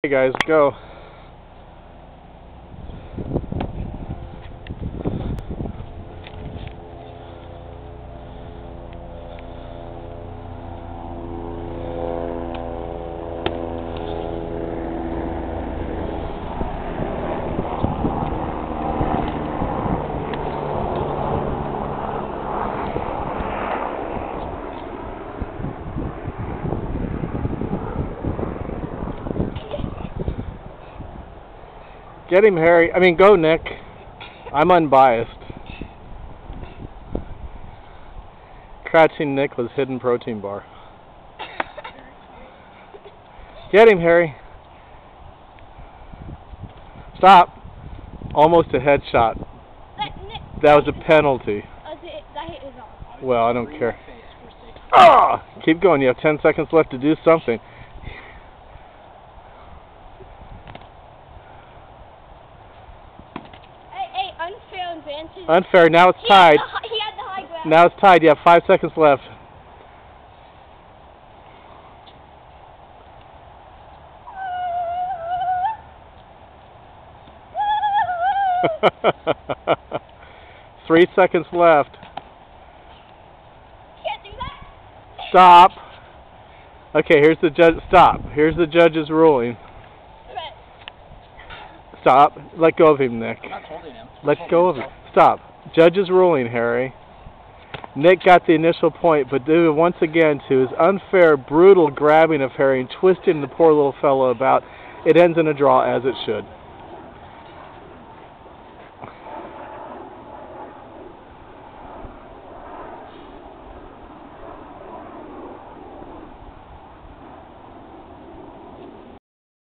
Nick and Harry battle
Nick the Unscrupulous find his exceptional sword skills sorely tested by the valiant Sir Harrison who, unlike Nick, is known for fair play and sportsmanship. Who will prevail?